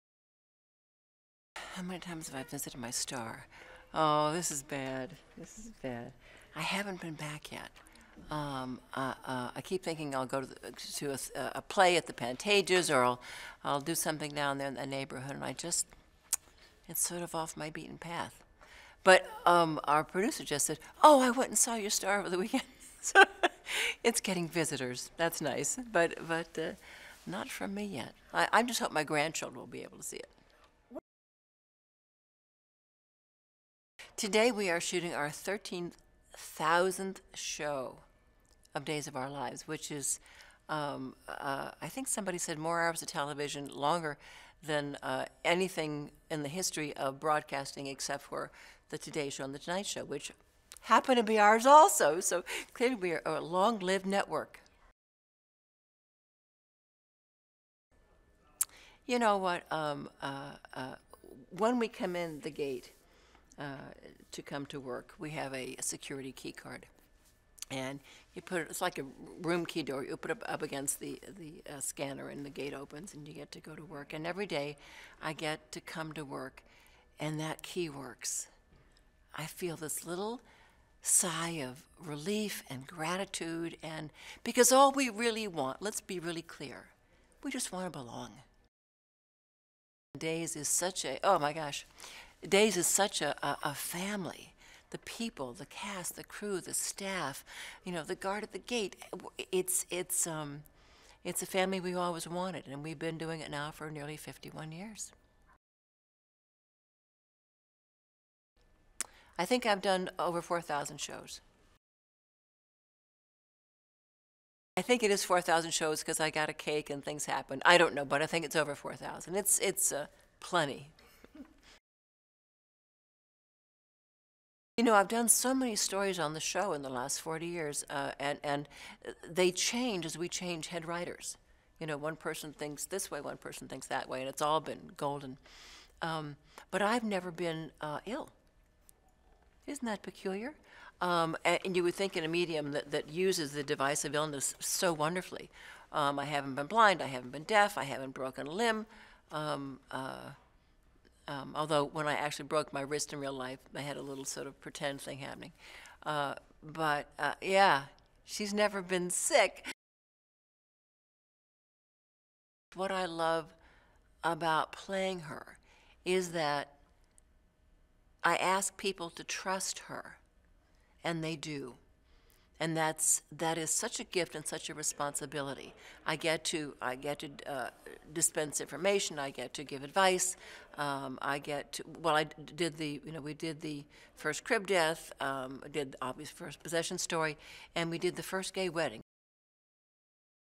Days of our Lives: 13,000th Episode || Deidre Hall Interview || SocialNews.XYZ
Watch Deidre Hall Interview about Days of our Lives: 13,000th Episode

ICONIC NBC DAYTIME DRAMA ‘DAYS OF OUR LIVES’ REACHES 13,000TH EPISODE

UNIVERSAL CITY, Calif. — July 19, 2016 — NBC’s longest-running daytime drama, “Days of our Lives,” is reaching yet another milestone with its 13,000thepisode, set to tape this week at the Burbank Studios.
The iconic series, which is in its 51stseason, will celebrate the occasion with a special “Days of our Lives” fan event Aug. 27 at the Wildhorse Saloon in Nashville, Tenn. There will be a Q&A and signing with talent from the show.
"This is truly a staggering achievement. We have 13,000 blessings to be thankful for today. My deep and abiding appreciation to NBC and Sony Pictures Television for their unwavering support throughout the decades,” said executive producer Ken Corday. “I am so grateful to the generations of artists, craftspeople and faithful viewers who have devoted much of their lifetimes to ‘Days of our Lives,’ falling in love with the people of Salem each and every episode. You have all made this breathtaking milestone possible."
“In an age of where too many shows come and go so quickly, the legacy and longevity of ‘Days of our Lives’ is absolutely remarkable,” said Jennifer Salke, President, NBC Entertainment. “We tip our cap to Ken Corday and his entire team, who have made the series an indelible part of television history.”
“Days of our Lives” first premiered as a half-hour drama in 1965 and expanded to an hour 10 years later. The series has garnered 46 Daytime Emmy Awards, including 2015’s Outstanding Daytime Drama, and numerous nominations, as well as multiple People’s Choice Awards, GLAAD Media Awards, and Prism Awards. The show airs nationally on NBC in the United States and in more than 25 countries internationally.
“Days of our Lives” is produced by Corday Productions Inc. in association with Sony Pictures Television. Ken Corday is executive producer, with co-executive producers Greg Meng and Albert Alarr. Dena Higley and Ryan Quan serve as head writers.